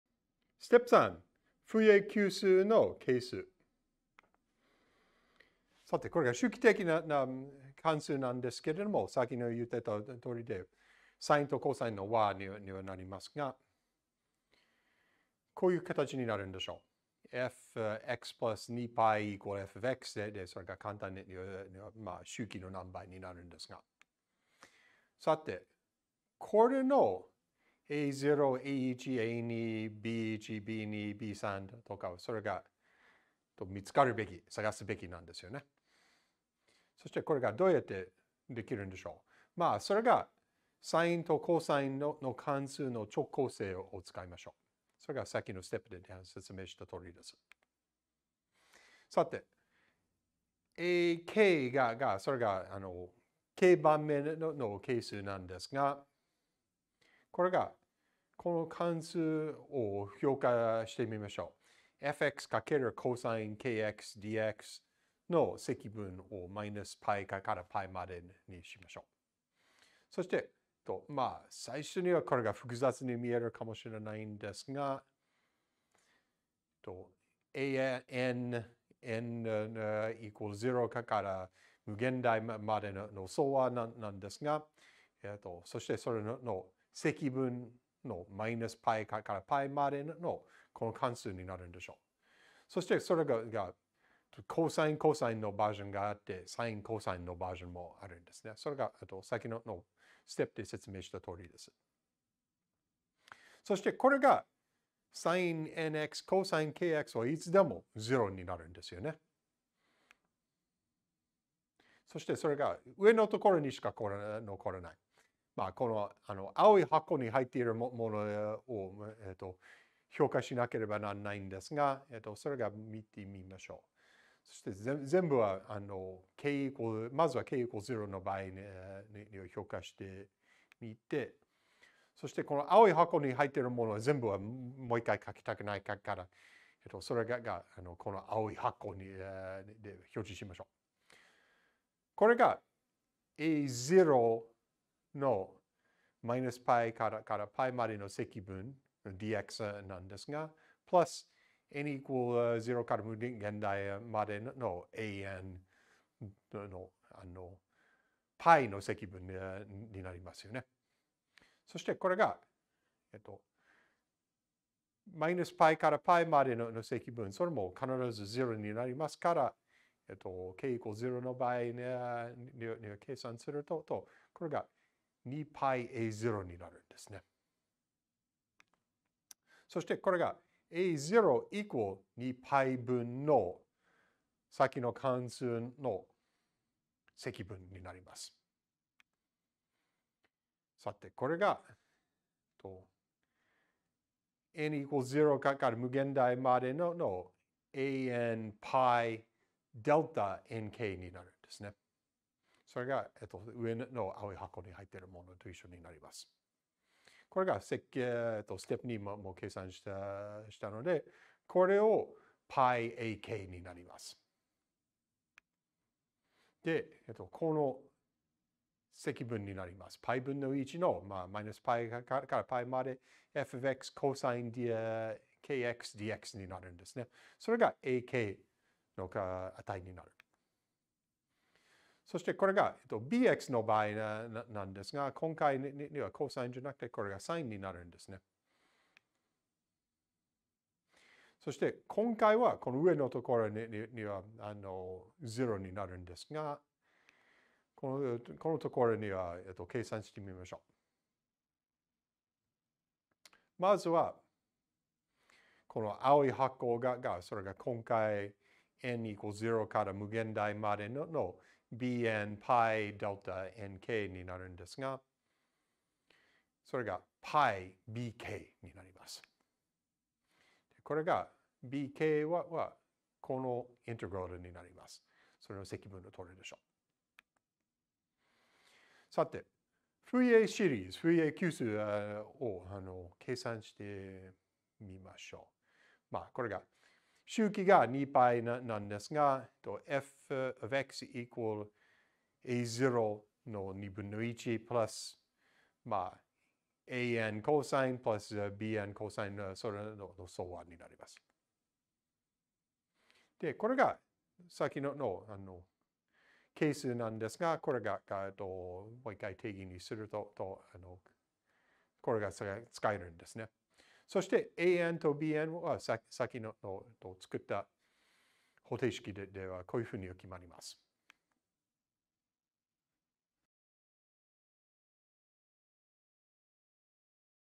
3-3 フーリエ級数の係数
レッスン 3 フーリエ解析 I

ステップ 3： フーリエ級数の係数

調和関数の直交性を使用してフーリエ級数係数を導出します。

本ビデオは「古典光学から量子光学へ」モジュールの一部です。